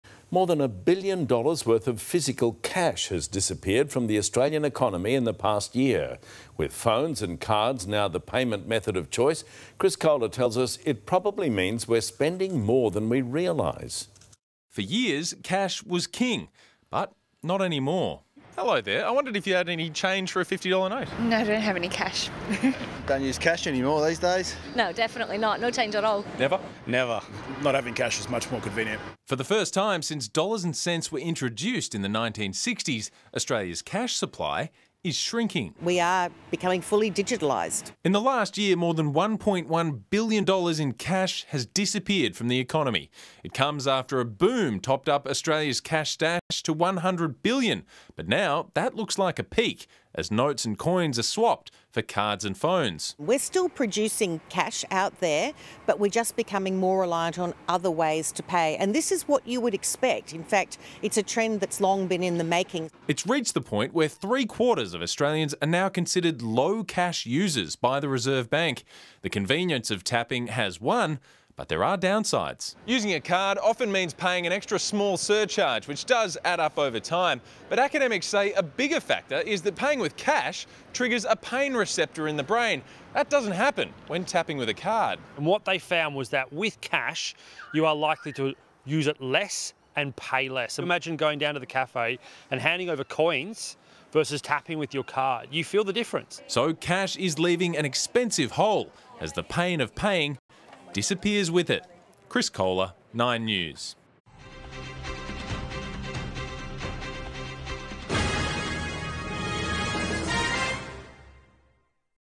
Australia could become totally cashless society | 9 News Australia
*Subscribe and 🔔 | It appears Australia is on its way to becoming a cashless society, with the number of notes in circulation officially declining for the first time since dollars and cents were introduced in 1966.